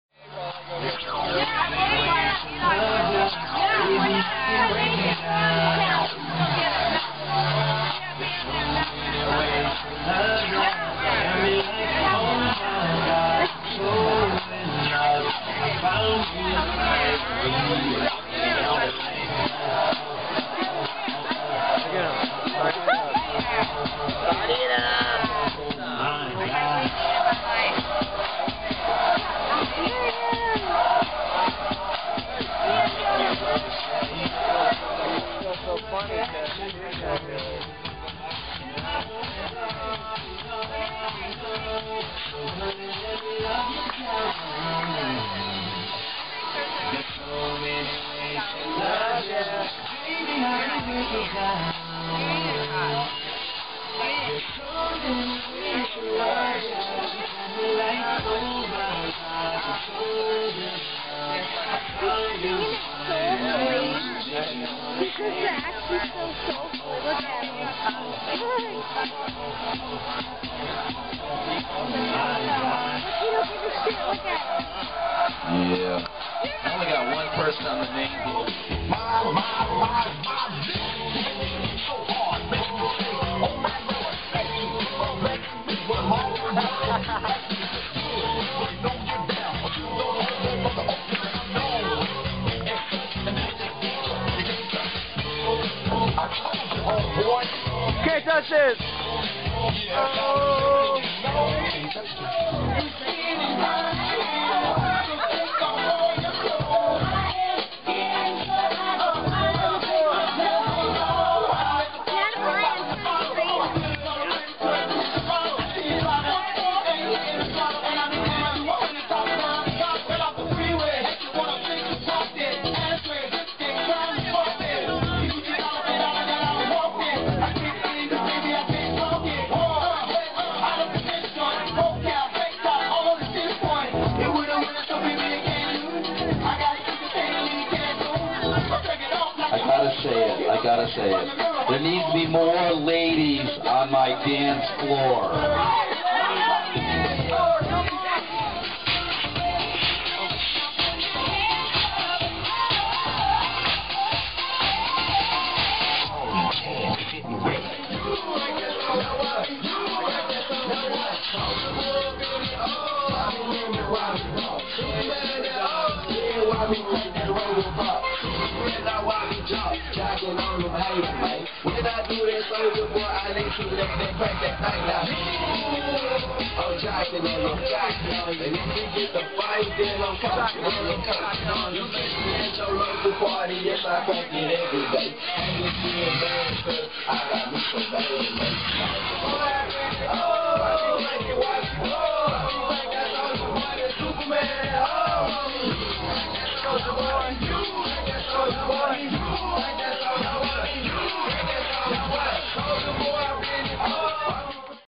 Brando Danceo (Daytona Beach Party it Up)
Brando Danceo (Daytona Beach Party it Up) White Boys Can Dance - LaPlaya Hotel, Brandon Dancing! 2010. BigBudde & BooBoo.
♥ BigBudde BooBoo CodyThePitbull PBnJBand ♥